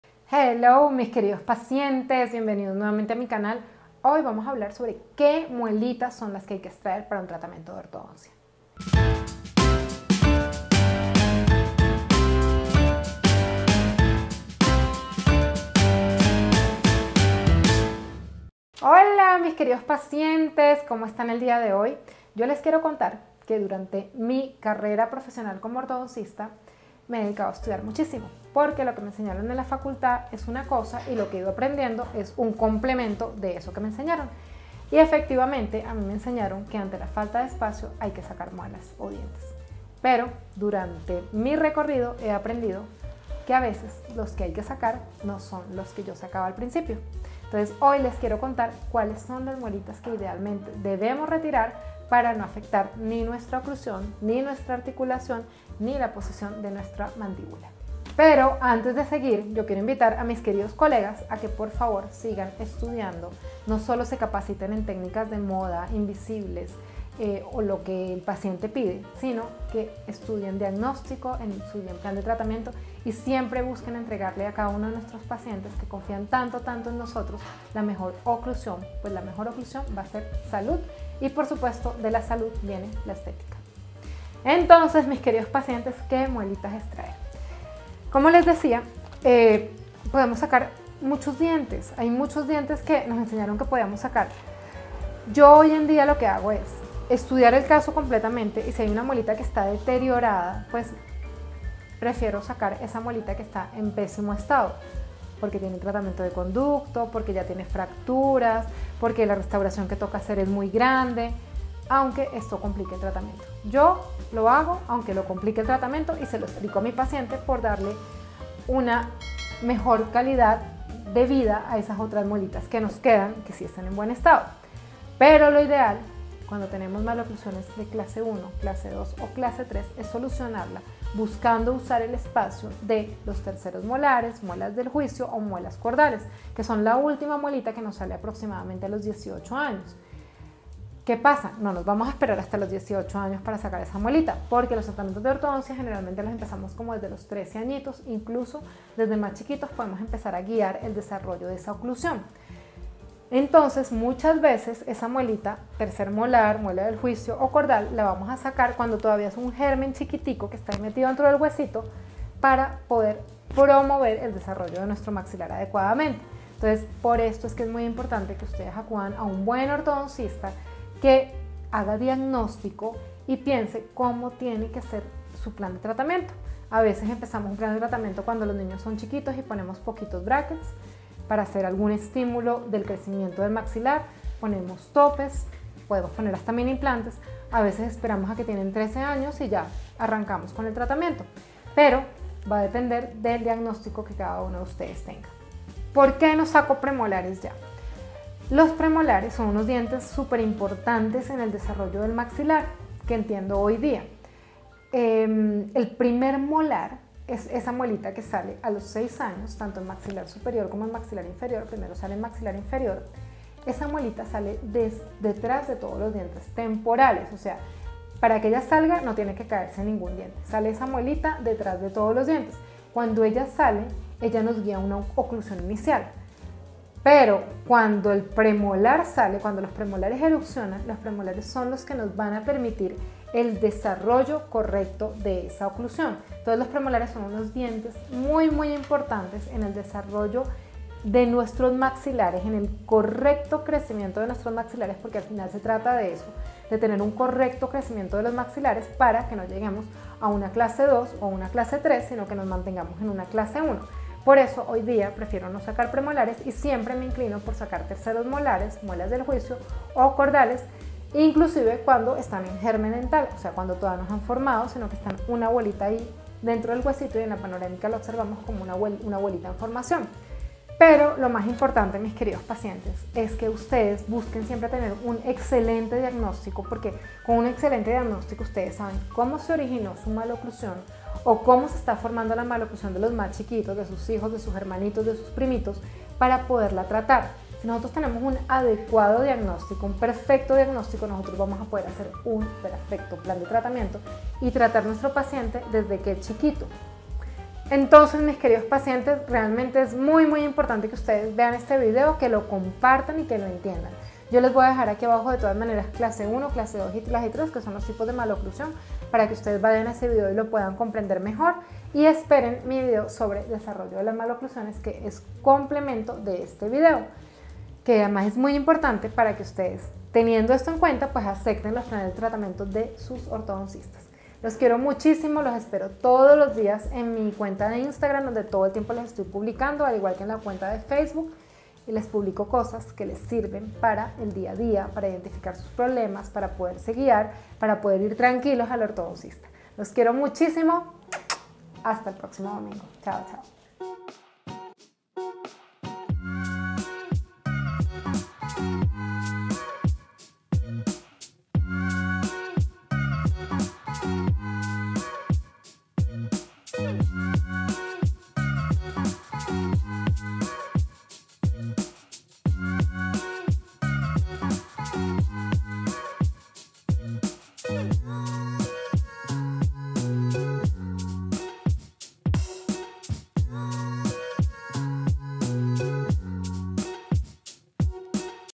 ¿𝐐𝐮é 𝐦𝐮𝐞𝐥𝐚𝐬 𝐄𝐗𝐓𝐑𝐀𝐄𝐑 𝐚𝐧𝐭𝐞𝐬 𝐝𝐞 𝐥𝐚 𝐎𝐑𝐓𝐎𝐃𝐎𝐍𝐂𝐈𝐀? ¡𝚢𝚊 𝚗𝚘 𝚜𝚊𝚌𝚘 𝚙𝚛𝚎𝚖𝚘𝚕𝚊𝚛𝚎𝚜!
Mis queridos pacientes y colegas, en este video les explico por qué ya no saco premolares antes de la ortodoncia y cuales son las piezas que prefiero eliminar antes de iniciar un procedimiento de ortodoncia, todo basado en un buen diagnóstico y un plan de tratamiento dental.

¿Quieres saber más? ve el video hasta el final y entérate de todo.

Videos Recomendados

Consultas: 8053784
Bogotá-Colombia